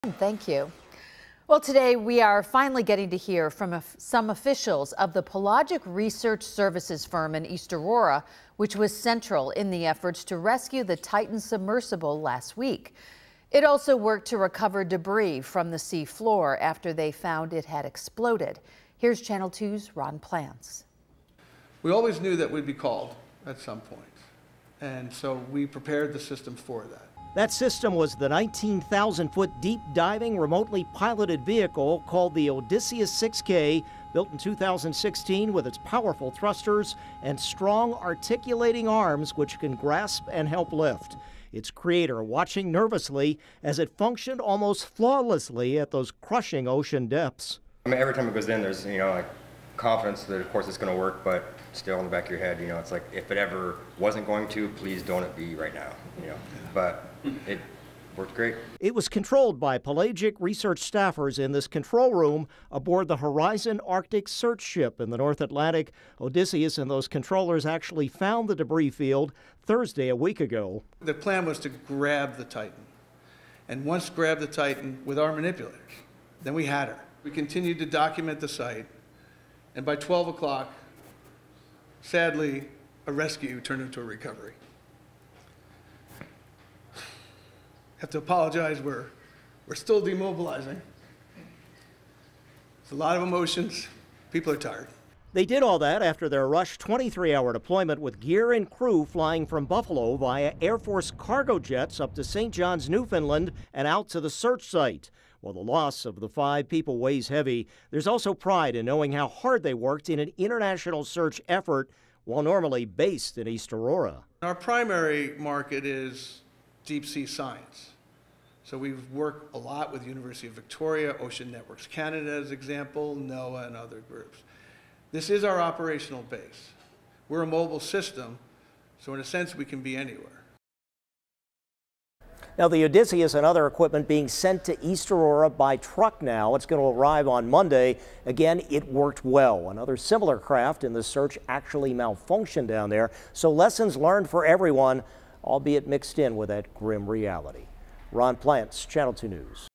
Pelagic end of mission
East Aurora company Pelagic on Titan search end of mission